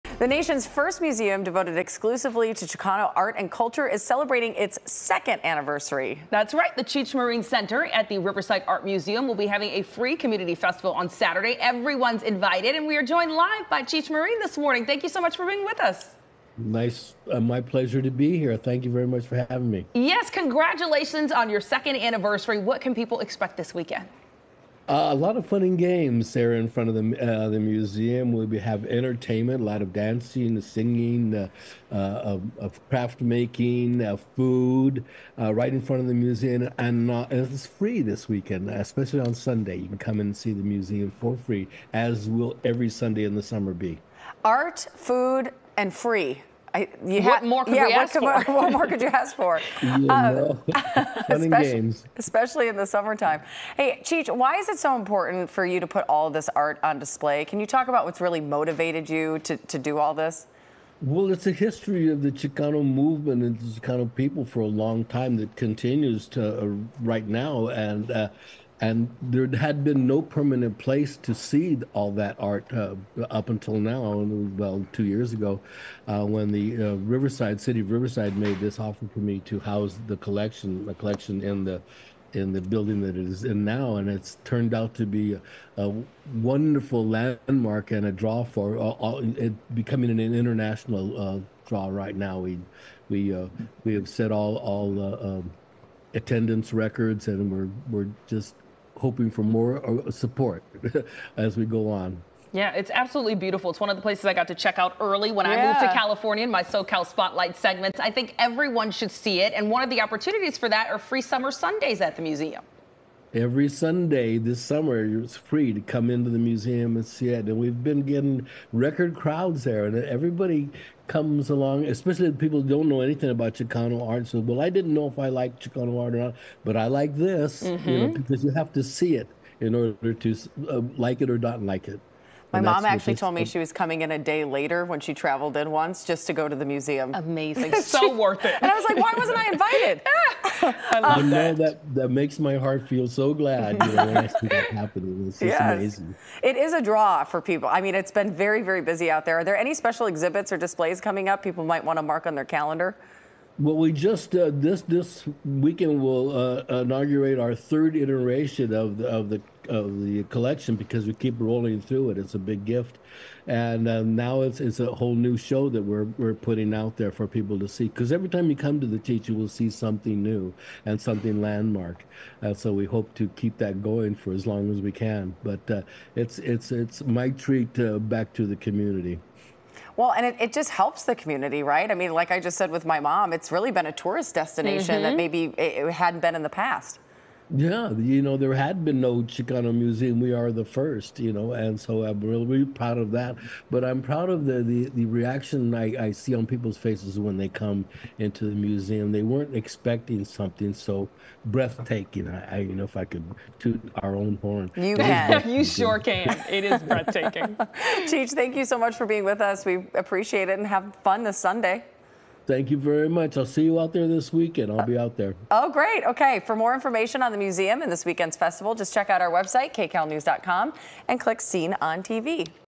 Cheech Marin talks about The Cheech celebrating its 2nd anniversary with a festival
The June 22 Arte Nuestra Community Festival at The Cheech art museum in Riverside takes place from noon to 7 p.m.